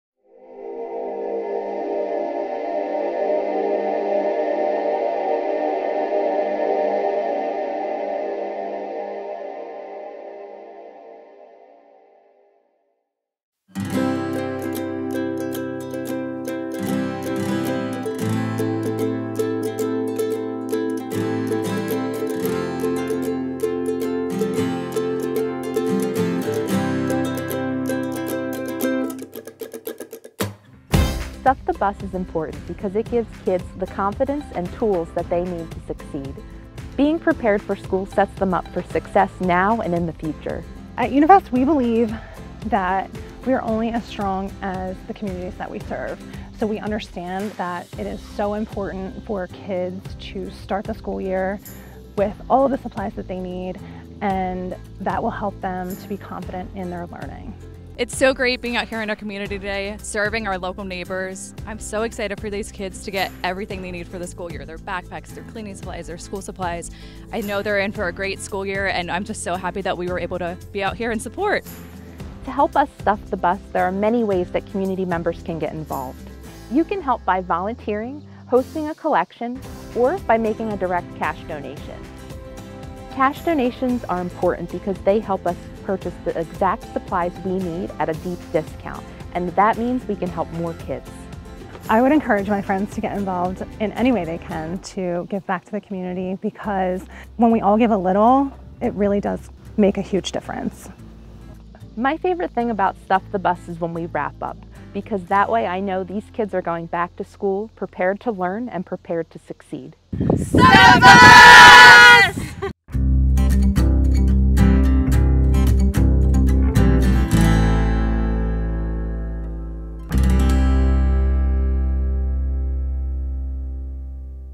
Stuff the Bus
Did you know that StufftheBus is the only community-wide school supply drive for local kids in need?

Each year, we serve over 3,000 students from every school district in Bucks County!

to fill hearts and brighten futures today.